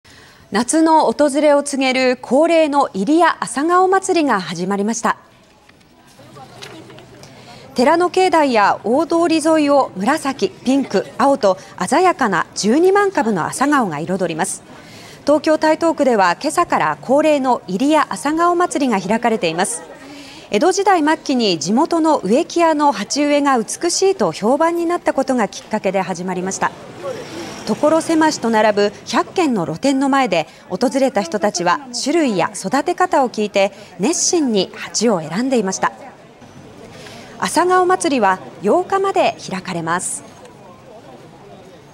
露店100軒に12万株 夏の訪れ告げる入谷朝顔まつり(16/07/06)
夏の訪れを告げる恒例の「入谷朝顔まつり」が始まりました。　寺の境内や大通り沿いを紫、ピンク、青と鮮やかな12万株の朝顔が彩ります。東京・台東区では6日朝から、恒例の入谷朝顔まつりが開かれています。